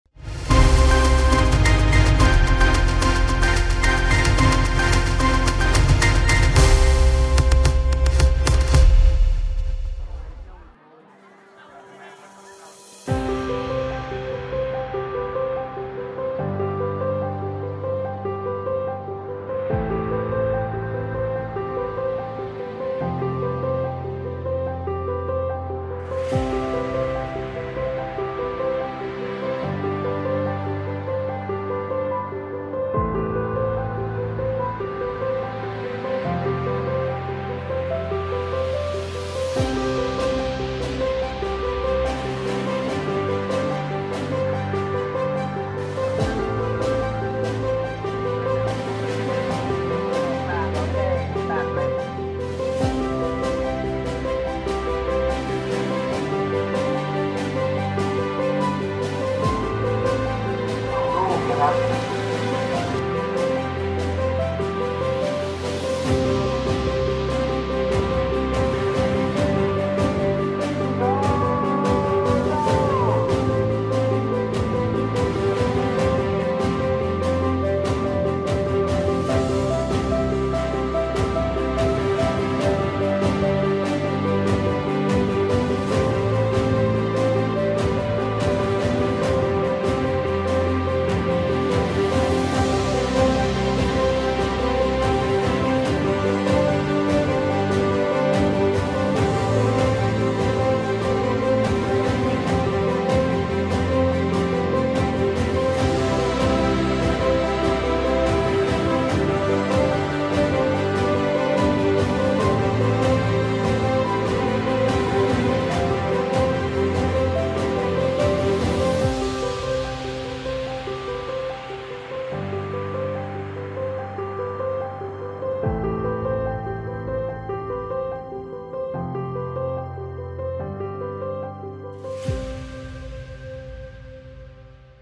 IMNA TV News
ပ္ဍဲကဵုကွာန်ဝင်က ဗလိုပ်ကျာ် ဗလိုပ်သင် ပ္ဍဲတ္ရဲကေၠာံသၞာံ